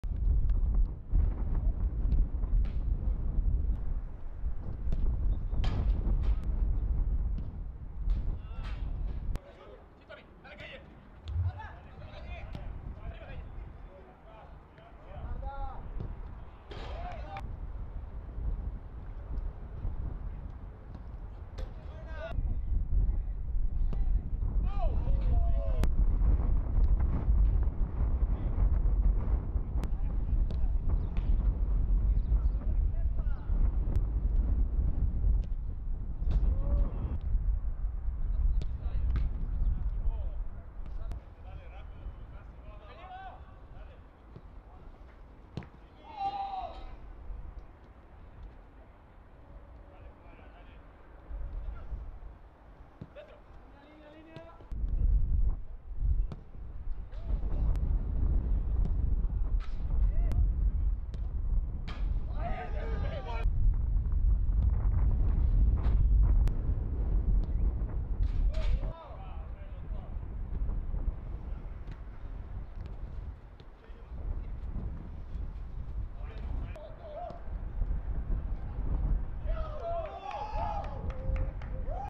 ¡CON JUDE BELLINGHAM INCLUIDO! EL ENTRENAMIENTO DEL REAL MADRID, PREVIO A SU DUELO CON VALENCIA
Real Madrid se preparó con todo, previo a su duelo ante Valencia en la Jornada 27 de LaLiga. Figuras como Jude Bellingham, Vinícius Jr., Toni Kroos, Luka Modric, Camavinga, entre otros, estuvieron presentes.

¿Cuánto quedará el partidazo entre Real Madrid y Valencia?

No te pierdas todos los partidos de la UEFA Champions League solo por Max